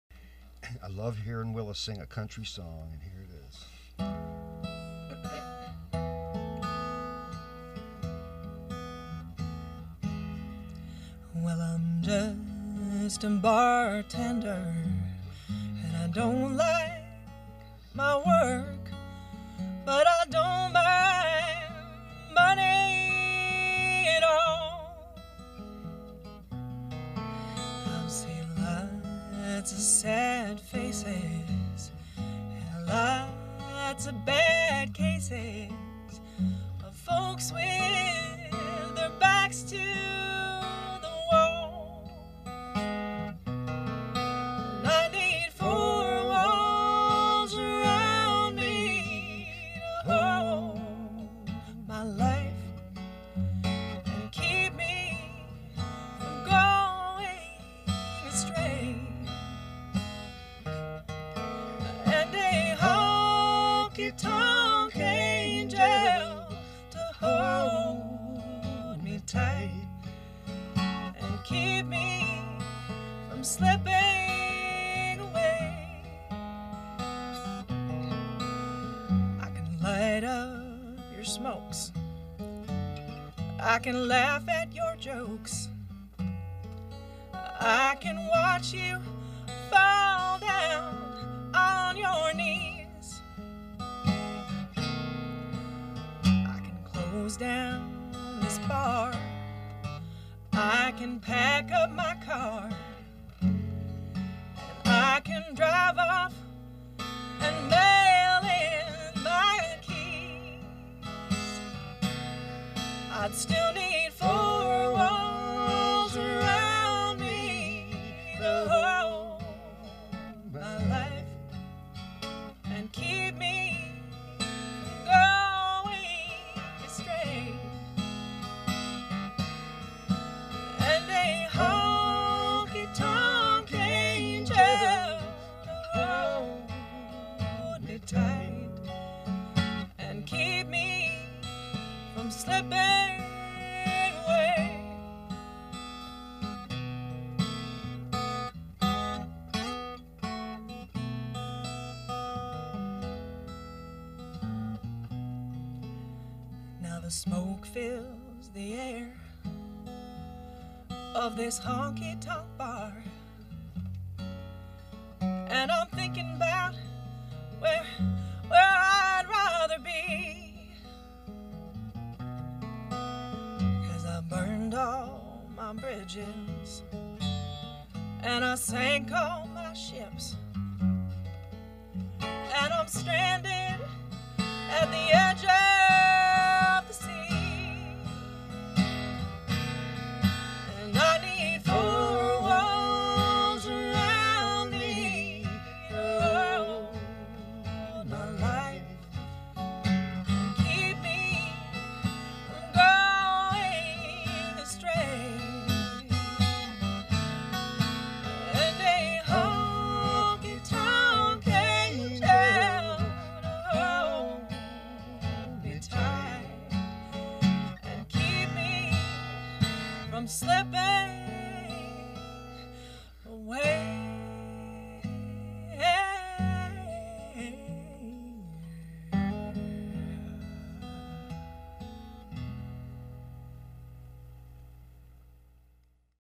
Willa Mamet and Paul Miller Perform on the Getaway
Last Thursday, Willa Mamet & Paul Miller stopped by the studio to play some songs and talk to Pitz about their upcoming shows. Watch their performance here.